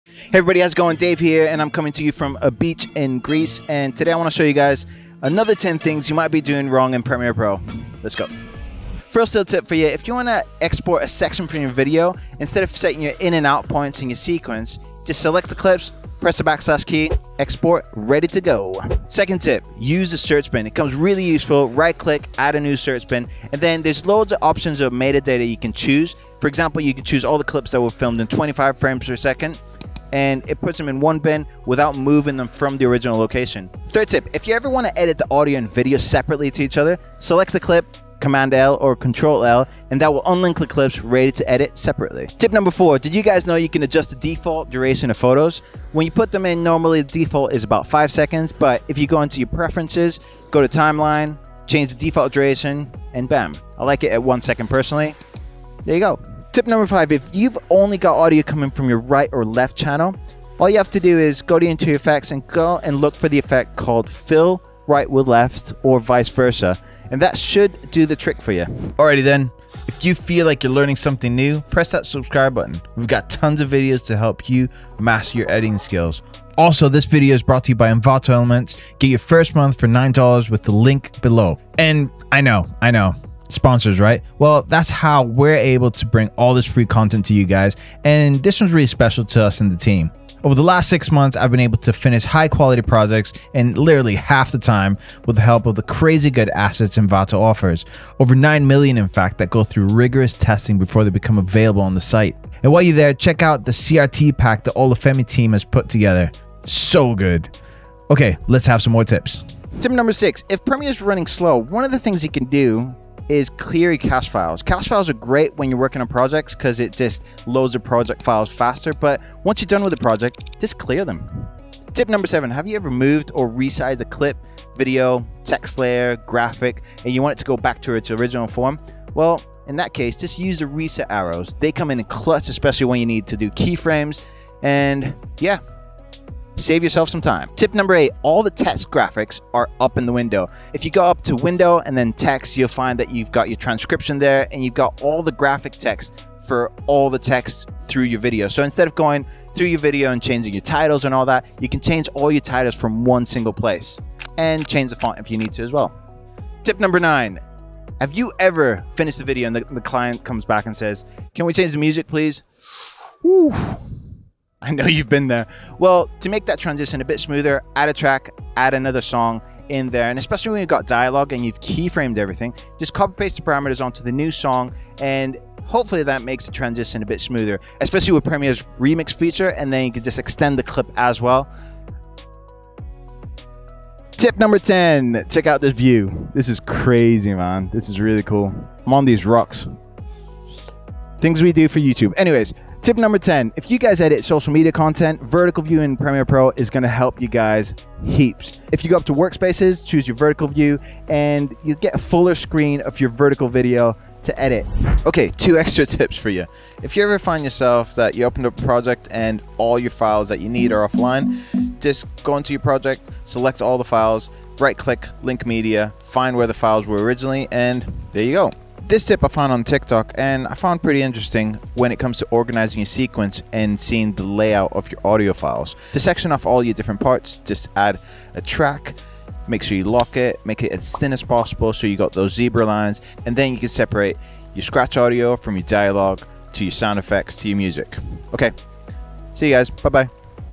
10 Premiere Pro Tricks Every Editor Should Know
In this video, you're going to learn 10 tricks that will help you get the most out of Premiere Pro. Tips that will speed up your workflow and help you improve your editing skills quickly.